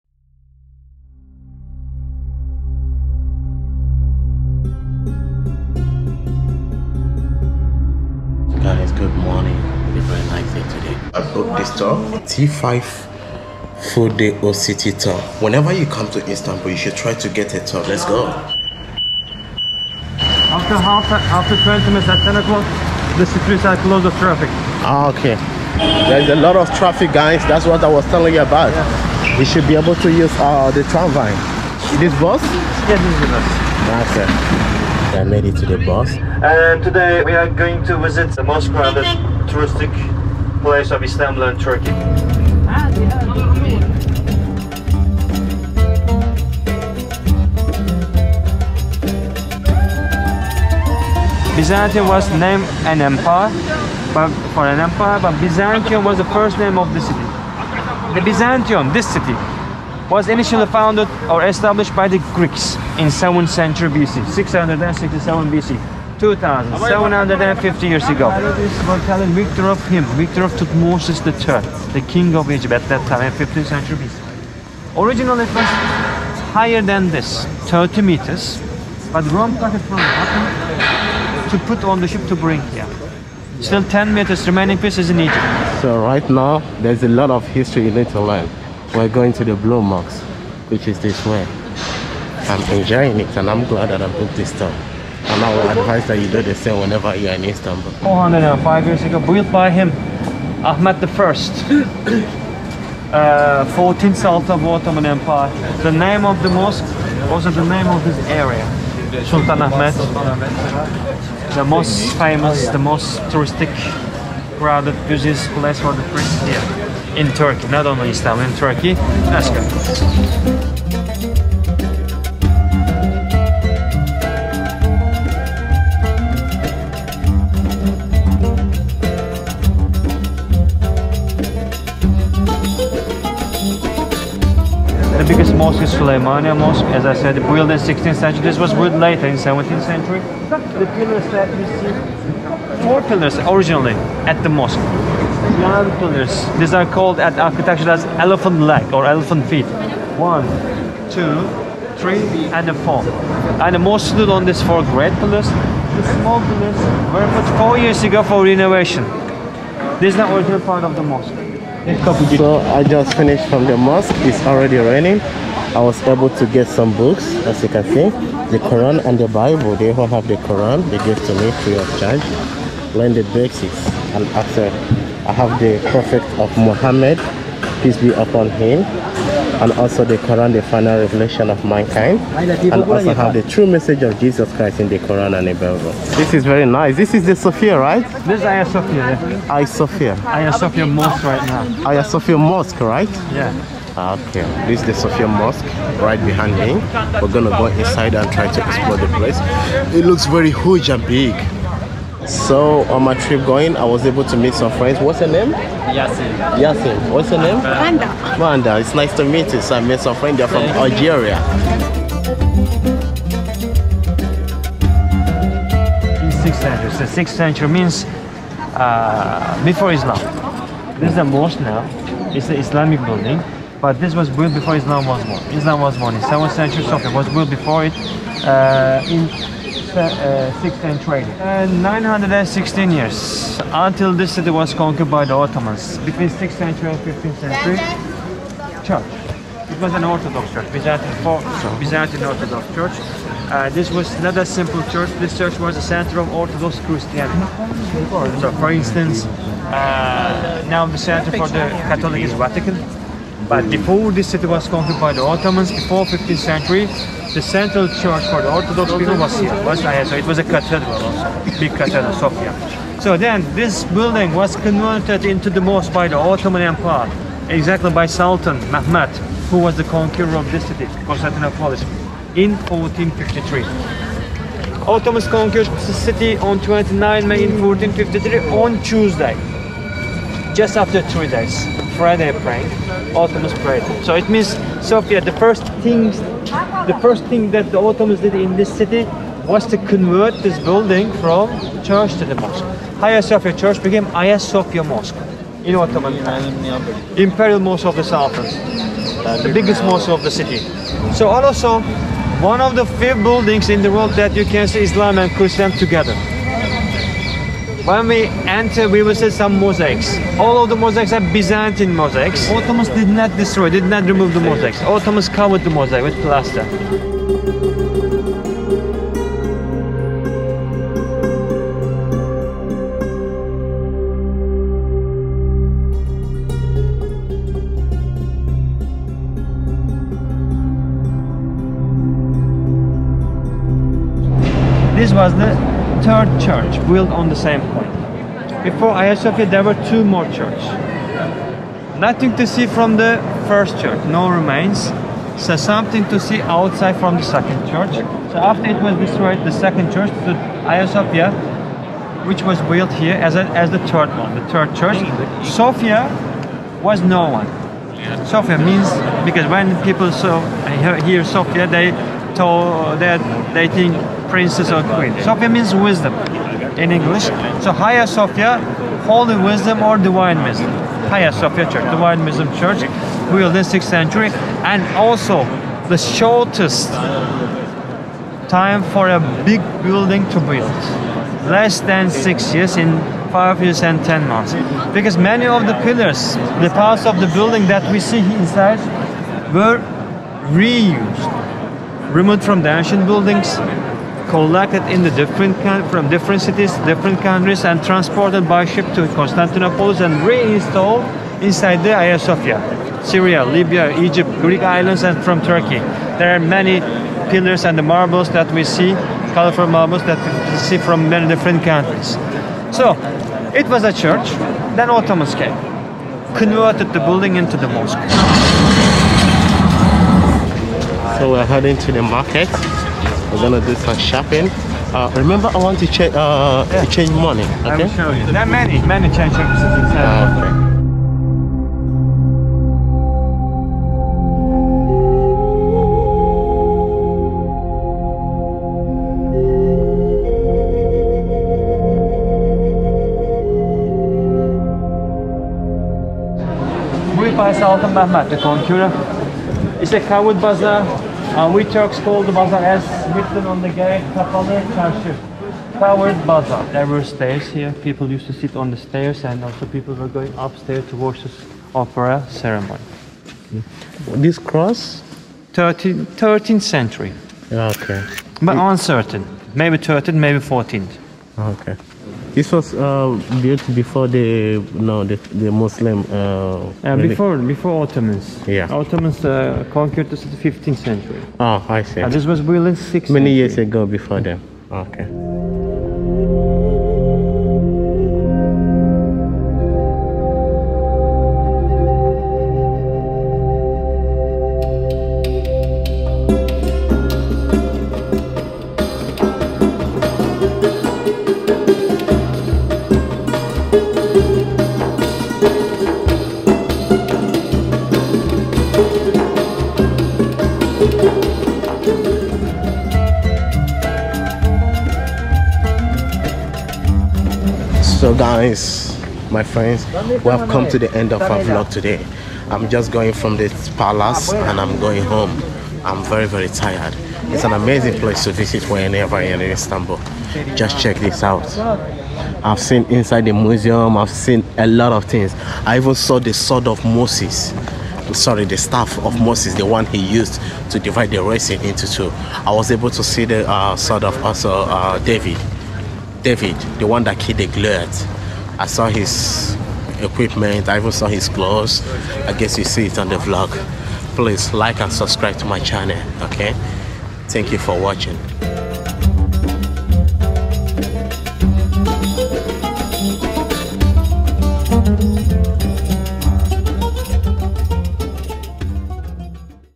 MOSQUE AND CHURCH IN THE SAME BUILDING.
In this vlog I visited the oldest city of Istanbul with a tour guide. We visited some  historical sites in the area, and learnt some historical facts. we went inside the Blue mosque that was convert from a church. This vlog is very educational and informative.
THE BLUE MOSQUE, HAGIA SOPHIA & TOPKAPI PALACE.
TAKING A TOUR OF THE OLDEST MOSQUE IN THE WORLD